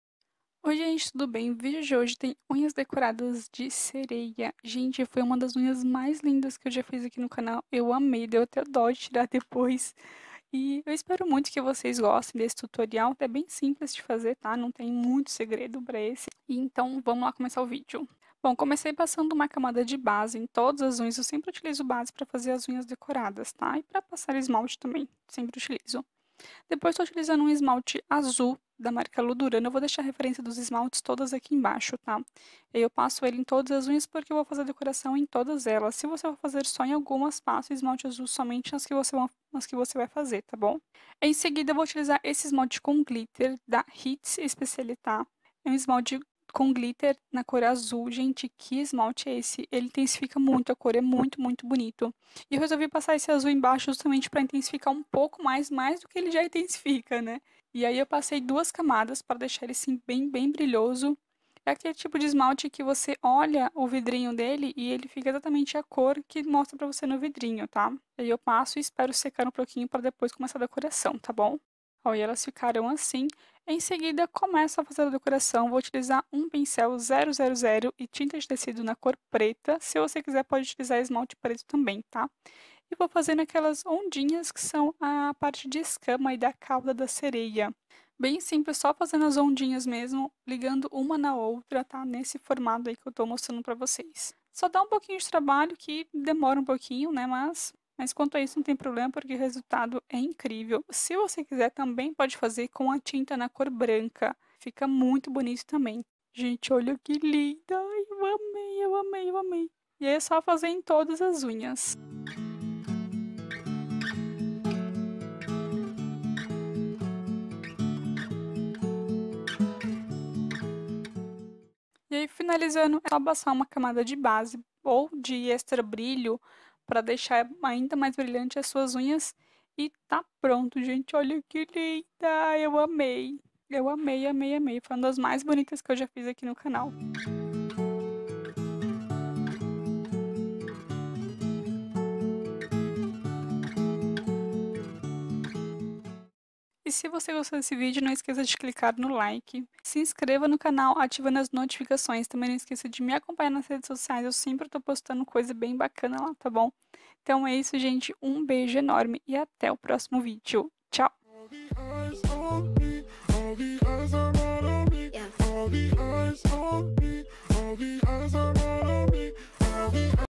Unhas DECORADAS DE SEREIA | Unhas Sereismo | Sil Soares
Não esqueça de ativar o 'sininho' 🔔 ok!?

❥ Produtos utilizados:
Esmalte base setim - colorama
Esmalta azul céu - ludurana
Esmalta 703 forte - hits
Esmalte máximo brilho - top beauty
Tinta de tecido preta
Pincel 000 - passi nati